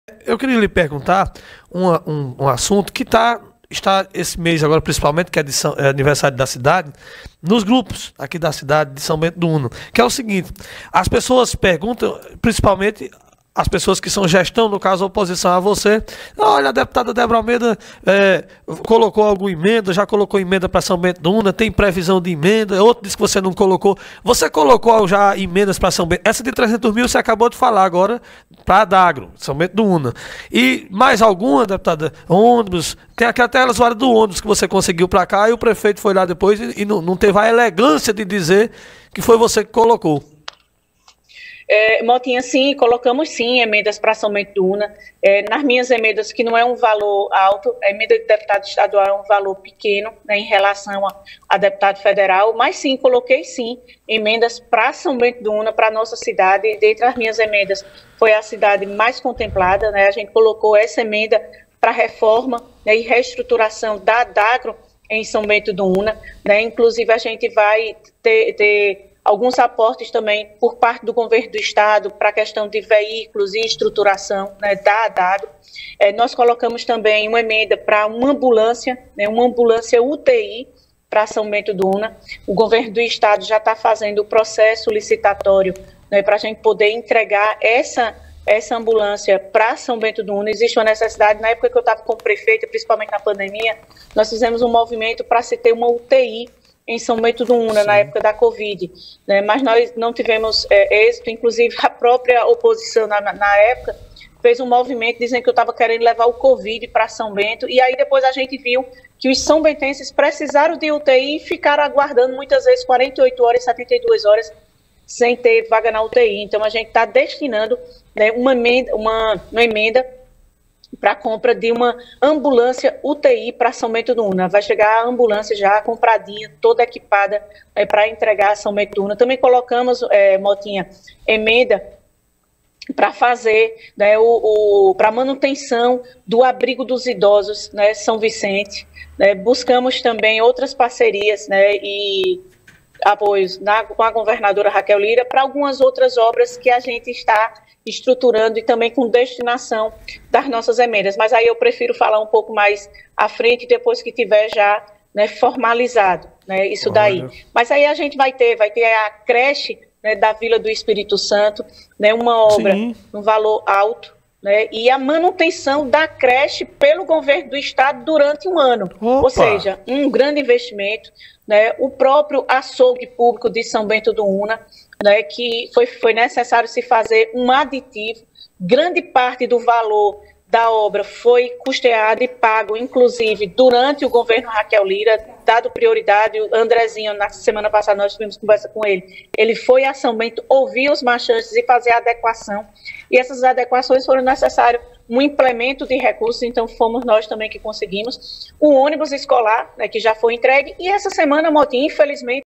Deputada Débora Almeida fala sobre as emendas que ela destinou para São Bento do Una
Deputada Débora Almeida fala sobre as emendas que ela direcionou para São Bento do Una

Em resposta a perguntas enviadas pelos ouvintes, nesta sexta-feira (26), a deputada estadual @deboraalmeida.pe falou sobre as emendas parlamentares que ela já destinou para a cidade de São Bento do Una, confira!